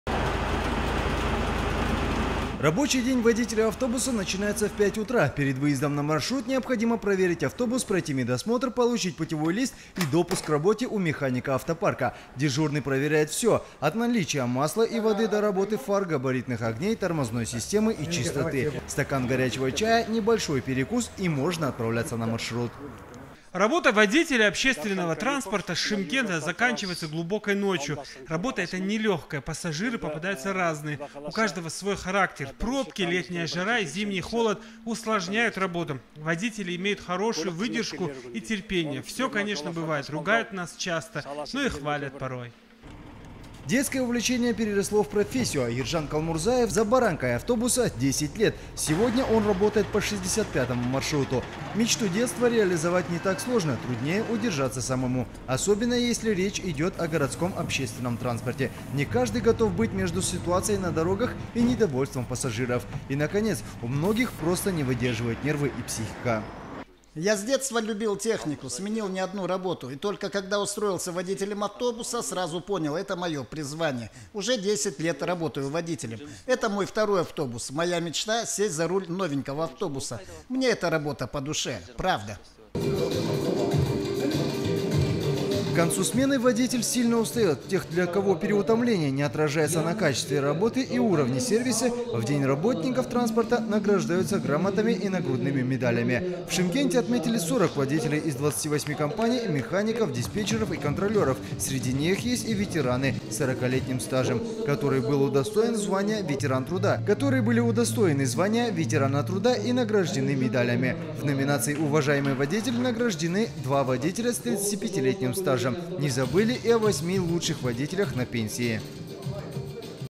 Профессиональный праздник в первое воскресенье августа отметят работники транспорта Казахстана
Несмотря на свой профессиональный праздник работать меньше пассажирские автобусы в третьем мегаполисе страны не станут.